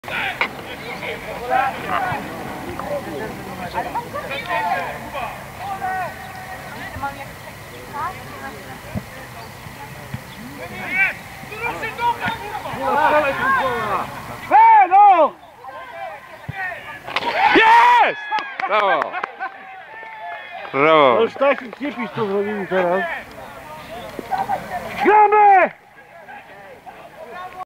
Prawobrzeże Szczecin - Gavia Choszczno 2:2
Bramkę dla Prawobrzeża strzela Oskar Tarnowski.